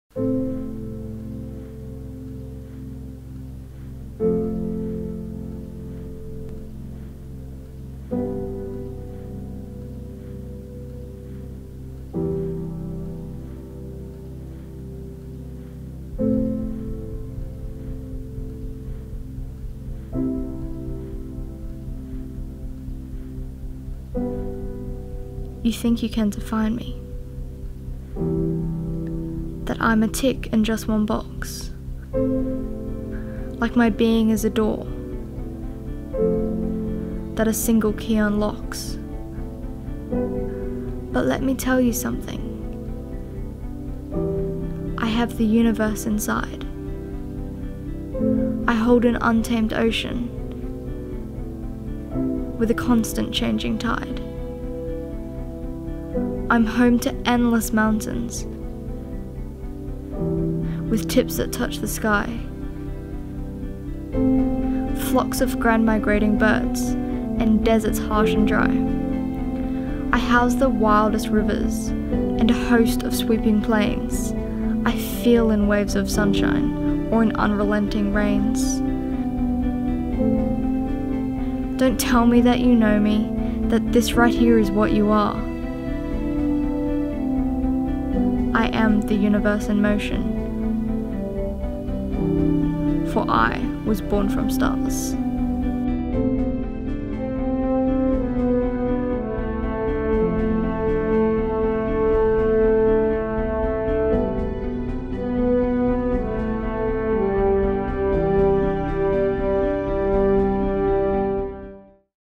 Short Cinematic Film | UNIQUELY UNIVERSAL (2018)
A little bit of an experimental short film about being yourself and not allowing others to define you. Based off a poem by Ernest Hemingway, a friend of mine and I went out to shoot some footage that I could play with in post production to experiment with different aesthetics, movement and filters. I hope y'all enjoy!

Directed by: Brooke Rankin (IG: @brookerankinfilms)
Written by: Ernest Hemingway
Edited by: Brooke Rankin
Actor: Piper Oliver (IG: @impiperoliver)
Soundtrack: Ross Bugden (IG: @rossbugden)